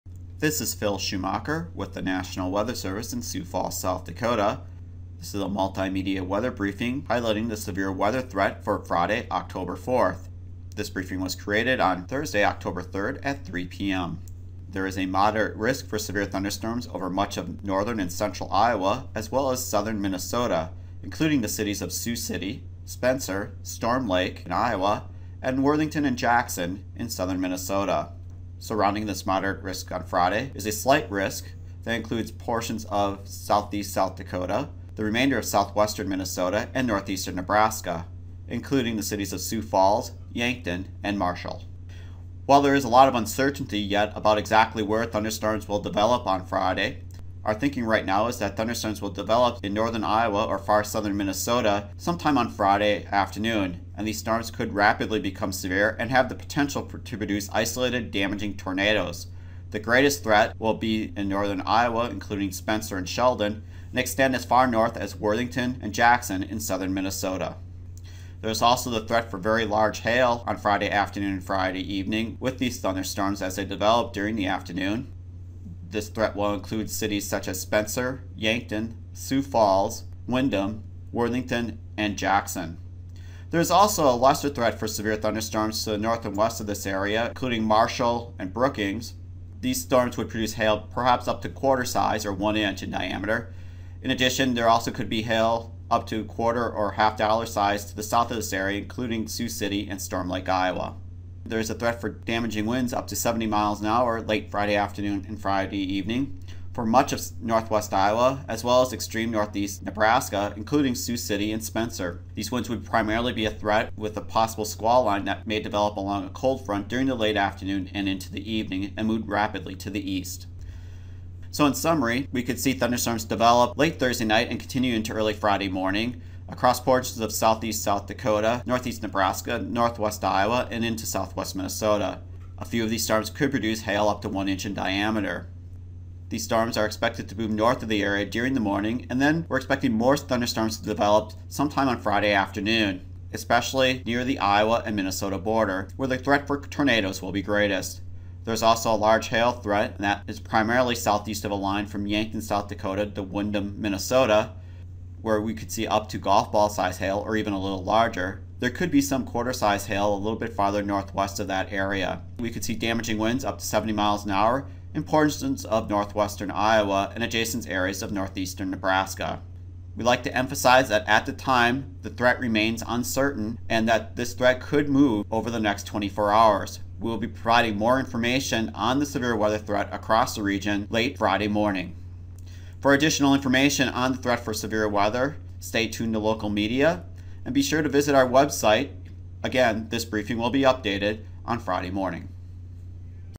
Multimedia Weather Briefing for the Severe Weather Threat on October 4, 2013
There is the possibility of isolated tornadoes, large hail and damaging winds Friday, October 4 over portions of northwestern Iowa, southeastern South Dakota, southwestern Minnesota and northeastern Nebraska.